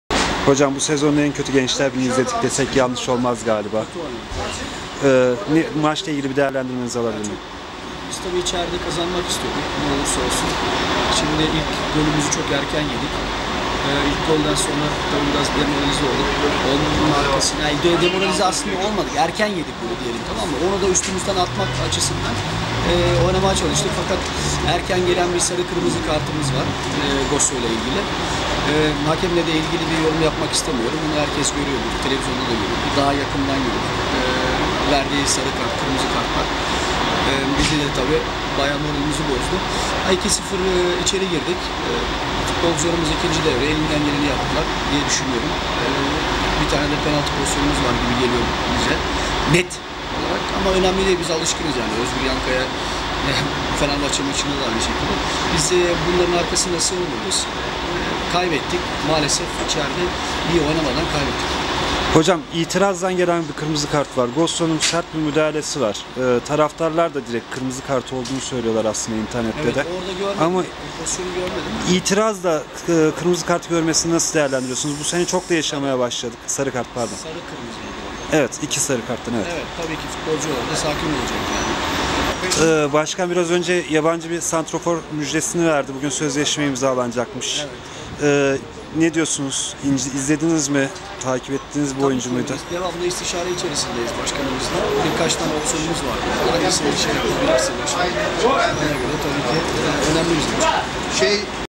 İrfan Buz'un Rizespor maçı sonrası değerlendirmesi...
Gençlerbirliği Teknik Direktörü 2-0 sona eren ve hakem Özgür Yankaya'nın 3 kırmızı kart gördüğü karşılaşma ile ilgili Klasspor'a yaptığı değerlendirme